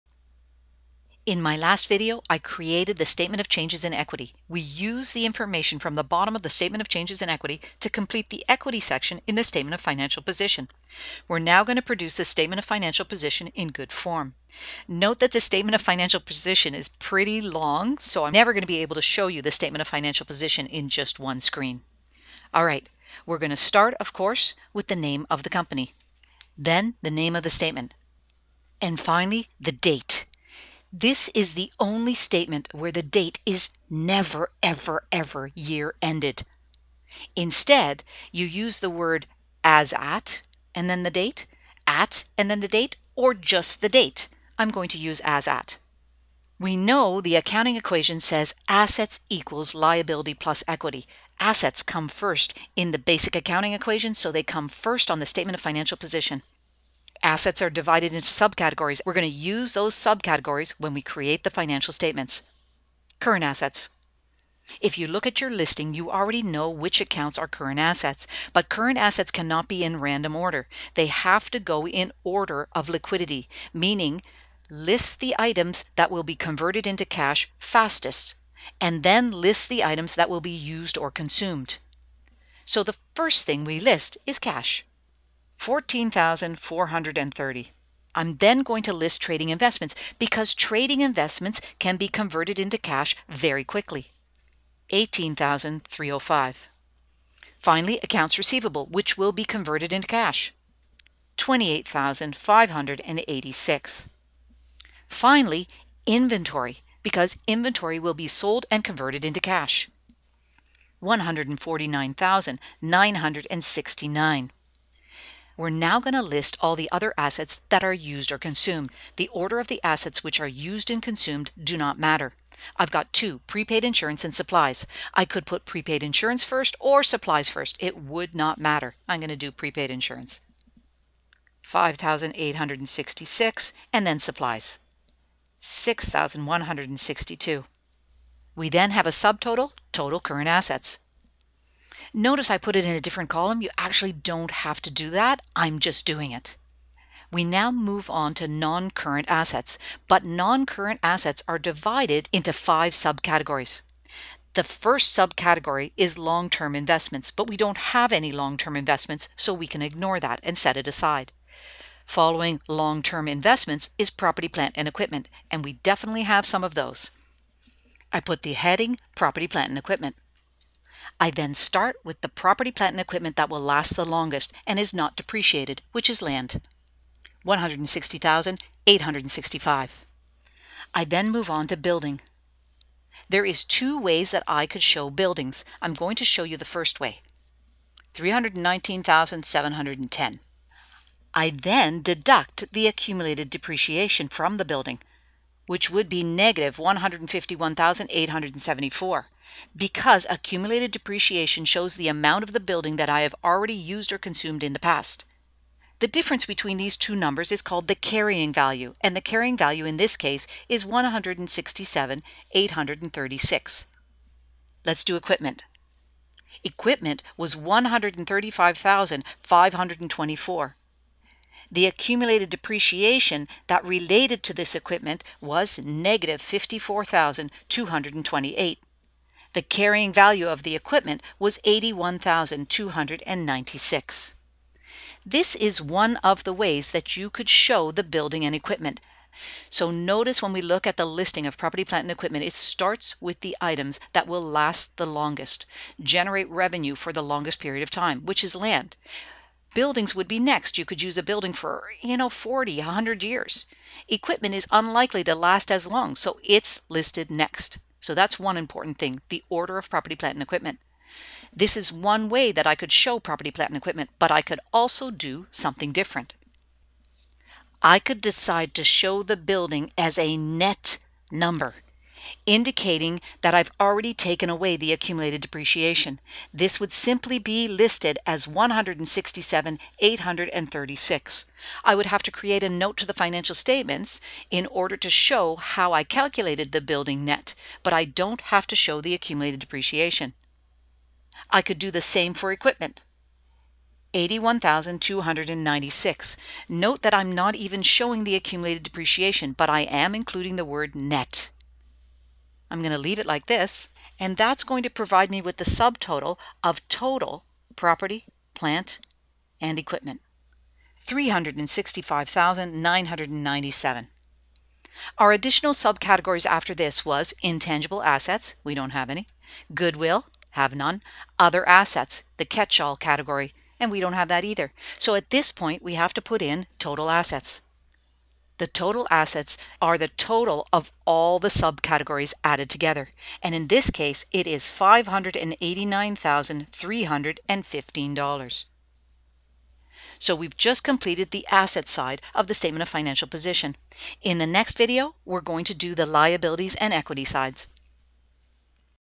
Module 2, F/S Problem FS-7, Part 5 - Statement of Financial Position, Assets
In this video I create the asset side of the statement of financial position.

Else